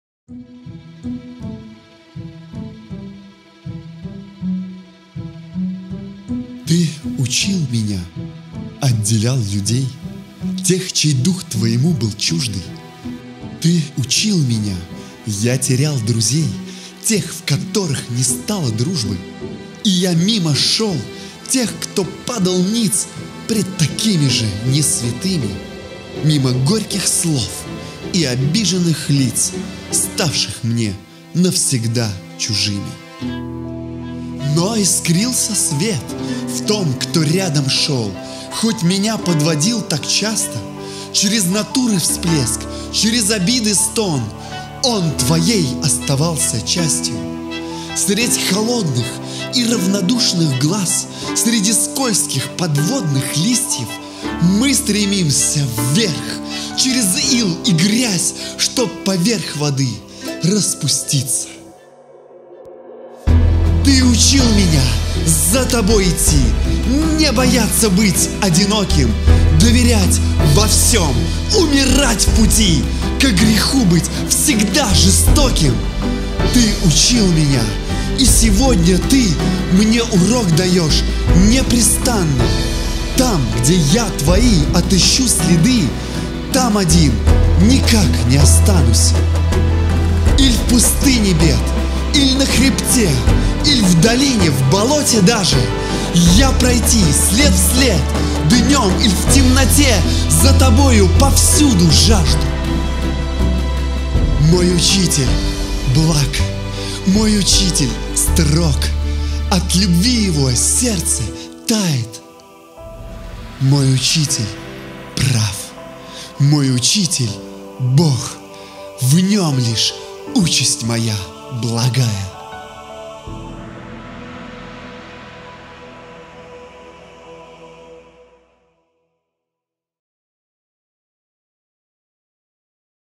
Ты учил меня - видеопоэзия - Лансере /стих/
Видео клип на стихотворение Наталии Лансере "Ты учил меня"
Музыка Наталии Лансере, читает Егор Лансере

Ты учил меня.… Отделял людей,
Тех, чей дух Твоему был чуждый.
Ты учил меня… Я терял друзей,
Тех, в которых не стало дружбы.

И я мимо шел тех, кто падал ниц
Пред такими же не святыми.
Мимо горьких слов и обиженных лиц,
Ставших мне навсегда чужими.

Но искрился Свет, в том, кто рядом шёл,
Хоть меня подводил так часто.
Чрез натуры всплеск, чрез обиды стон –
Он Твоей оставался частью.

Средь холодных и равнодушных глаз,
Среди скользких подводных листьев,
Мы стремимся вверх, через ил и грязь,
Чтоб поверх воды распуститься.

Ты учил меня за Тобой идти,
Не бояться быть одиноким.
Доверять во всем, умирать в пути,
Ко греху быть всегда жестоким.

Ты учил меня…. И сегодня Ты
Мне урок даешь непрестанно:
Там, где я Твои отыщу следы,
Там один никак не останусь.

Иль в пустыне бед, или на хребте,
Иль в долине, в болоте даже,
Я пройти след в След, днем иль темноте,
За Тобою повсюду жажду.

Мой Учитель благ, мой Учитель строг,
От любви Его - сердце тает…
Мой Учитель прав, мой Учитель - Бог,
В Нём лишь участь моя благая